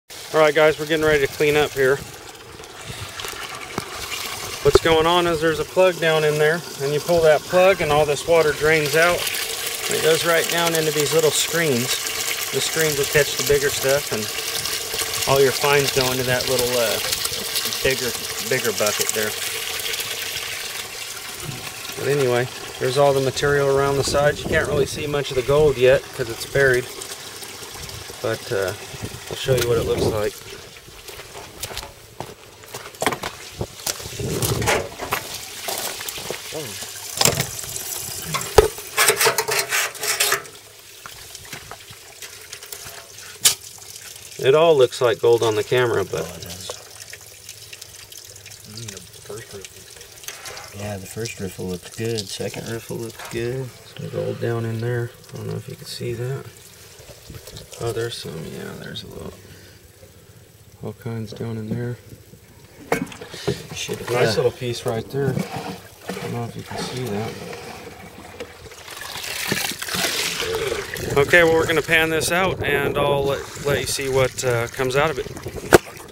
Gold Mining - Gold Clean Up Concentrator 2 - The Centrifuge
This is a gold concentration machine called a gold centrifuge.It can do up to 5 yards an hour of minus 1/4" material. Works very very well but does not capture small minus 50 gold very well.